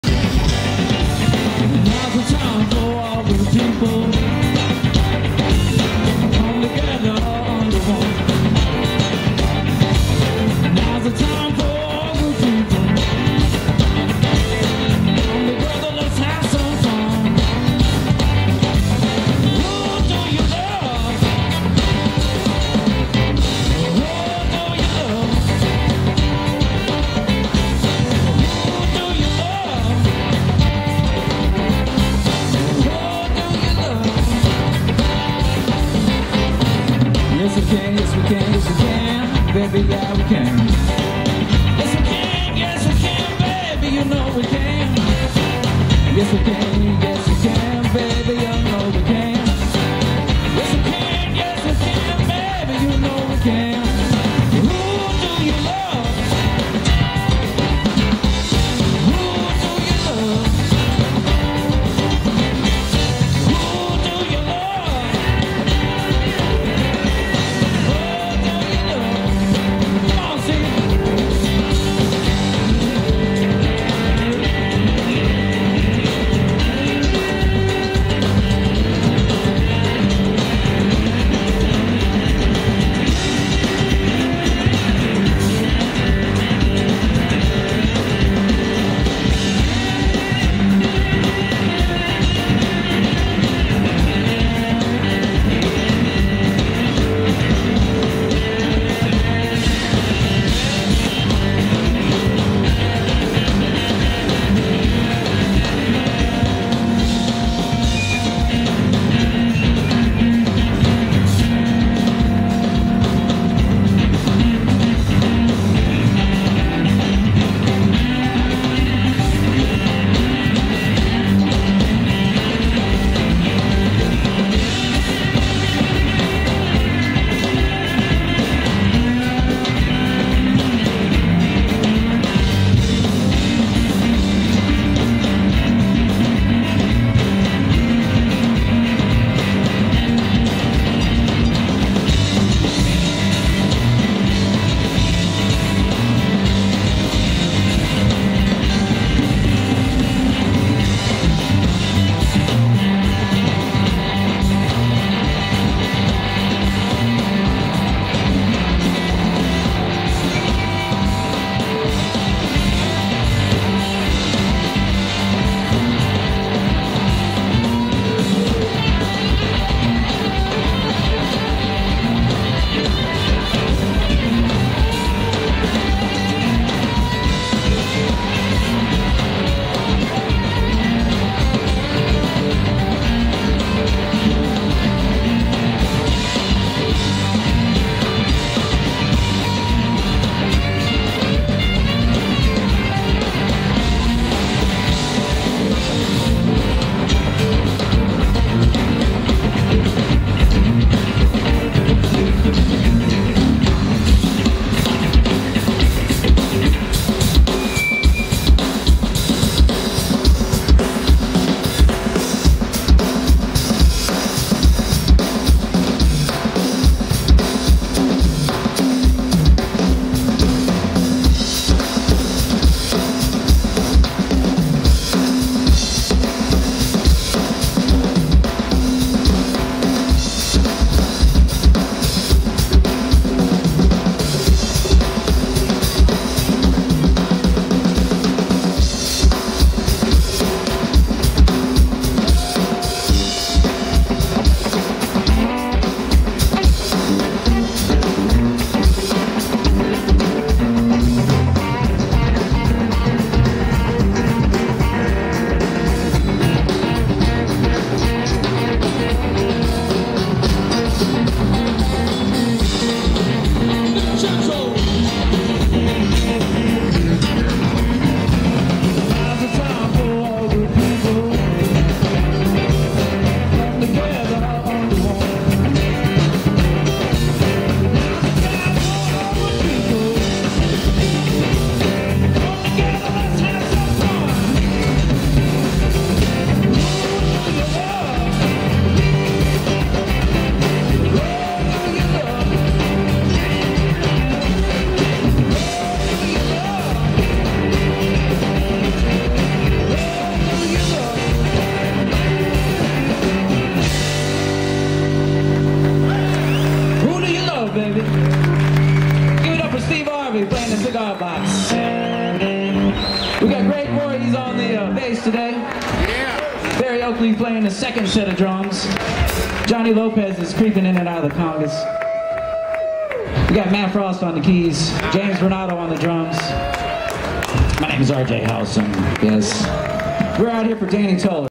RJ Howson Live at the Sun King Festival 2012 Steve Arvey Cigar Box Guitar
Sun King Festival April 7th 2012  Rj Howson and Steve Arvey Matthew Frost Keyboards Greg Voorhees on Bass James Varnado and Barry Oakley Jr. Drums  VIDEO SHOT BY STEVE TOMCZAK